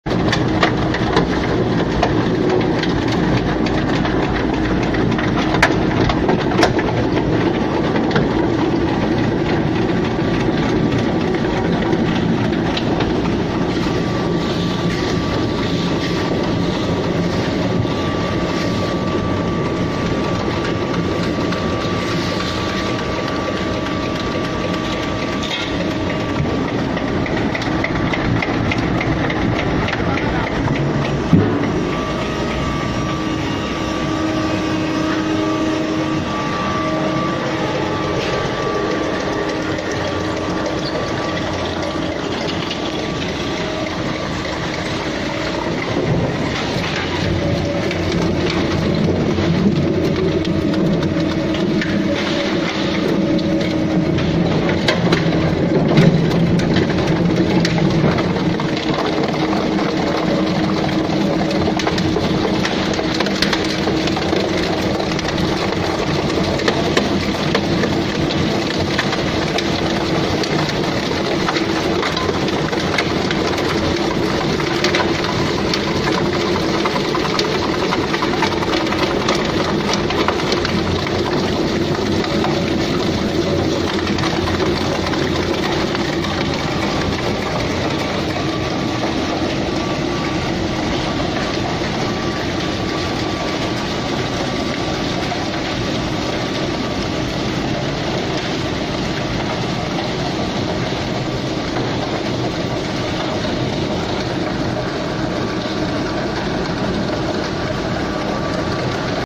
plastic pipe shredder decreases hdpe pvc ppr waste
plastic pipe shredder cuts large and heavy wall plastic waste automatically
heavy duty single shaft shredder machine designed for long and big plastic lumps and plastic pipes , 1-3 meters plastic pipe can be feeded into the hopper of the heavy duty single shaft shredder directly.
waste plastic pipe lumps shredder with single shaft in the hopper, hopper pushes plastic pipe and lumps onto the rotating blades ,and being converted into small chips for reuse
The Hdpe Pipe Shredder is basically an industrial single shaft shredder that has a vertical hopper to store big and long hdpe plastic pipe . The vertical hopper performs the pushing function to make the large diameter hdpe plastic pipe shredded in full sectional area.
The hdpe pipe shredder consists of hopper-pushing system which is controlled by hydraulic station , and shredding system which includes motor ,gearbox , round rotor, and square blades with 4 angles.